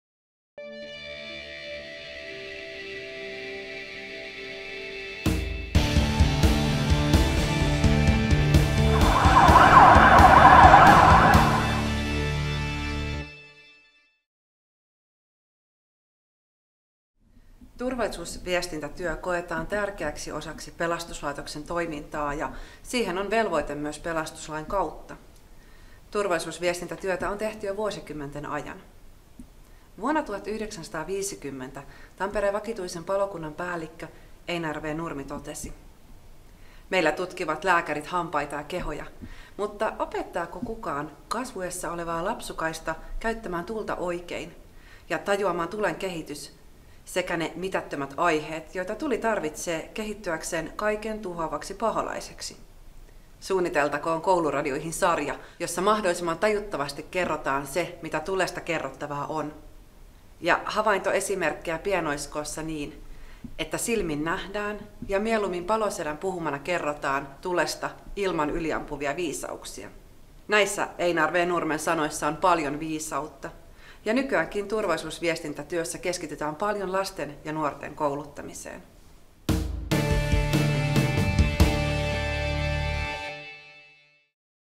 14. Palosedät valistakoot - 120 tarinaa Pirkanmaan pelastuslaitokselta
Palopäällikkö Einar V. Nurmi kantoi huolta jo 1950-luvulla, opettaako kukaan kasvuiässä olevaa lapsukaista käyttämään tulta oikealla tavalla. Hänen mielestään oli tärkeää, että nimenomaan palokunta kertoo tulipalon vaaroista. Nykyäänkin turvallisuusviestinnässä keskitytään paljolti lasten ja nuorten kouluttamiseen.